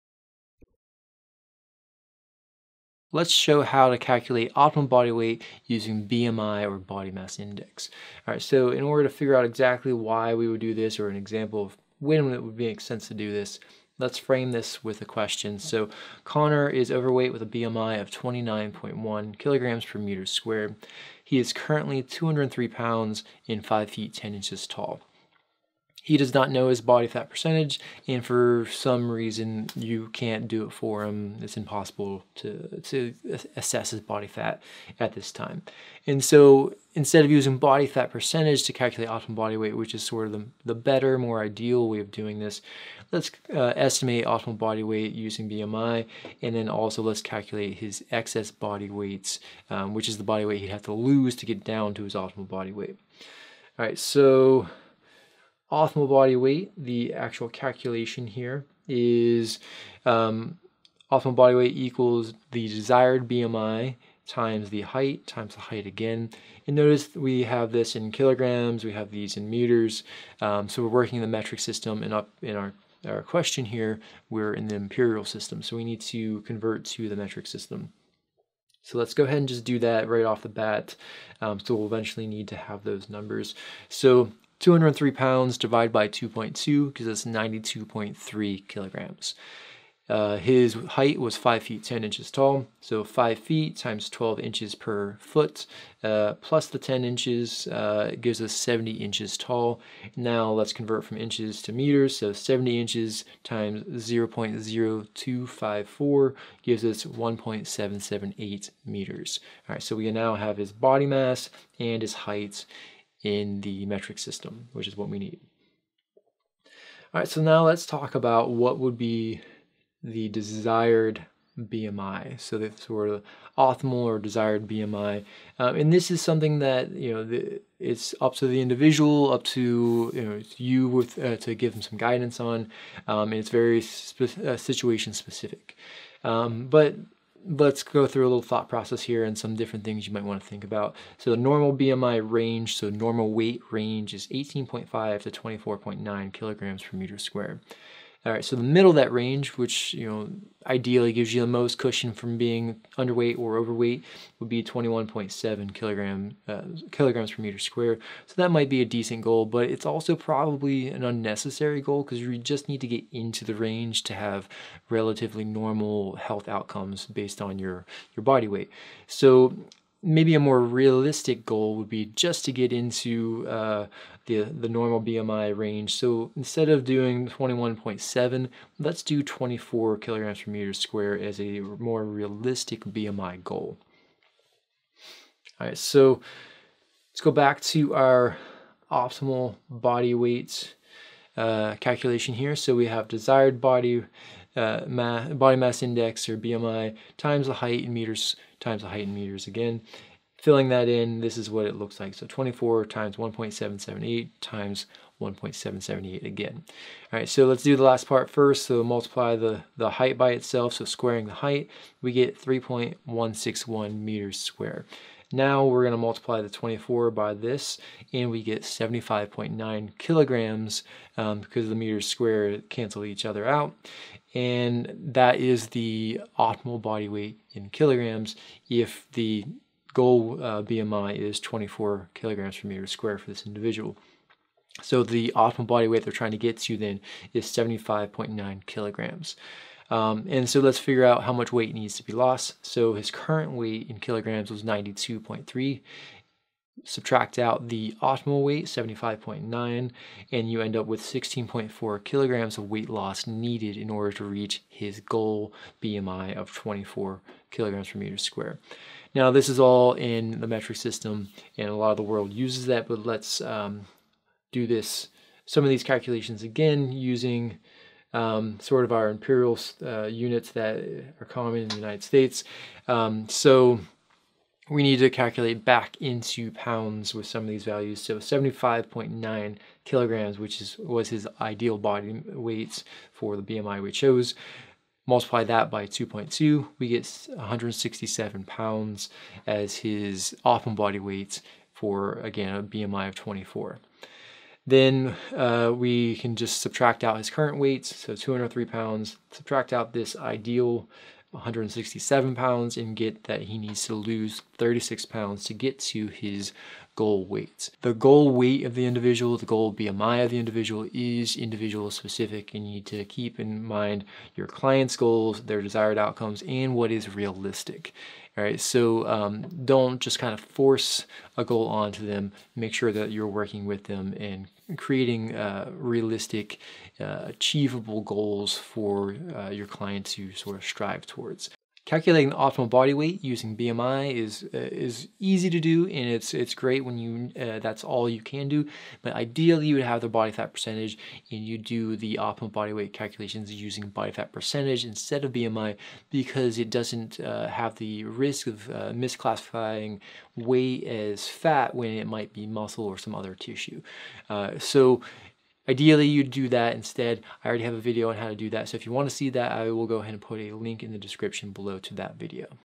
Optimal Body Weight Calculations by BMI
This video shows Dr. Evan Matthews providing a brief overview of how to calculate optimal body weight based on body mass index (BMI). This can be done when body fat percentage is not known.